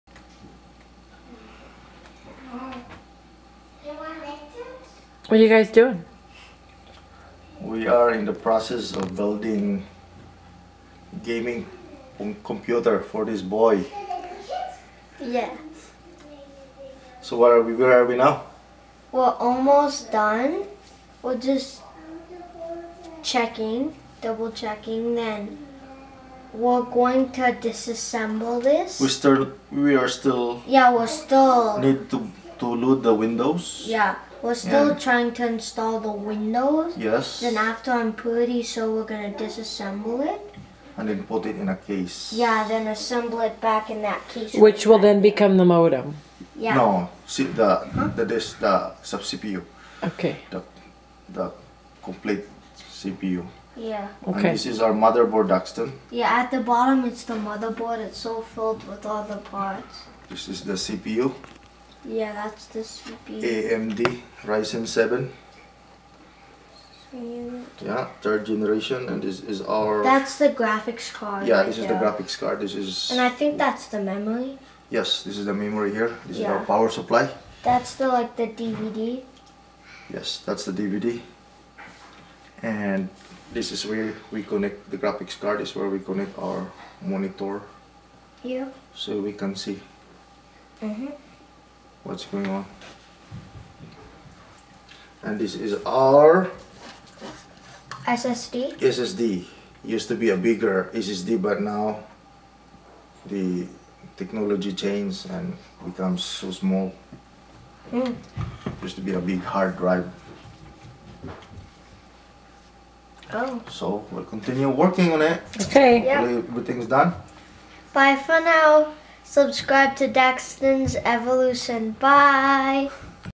Testing the PC components
Testing the PC components before putting in the case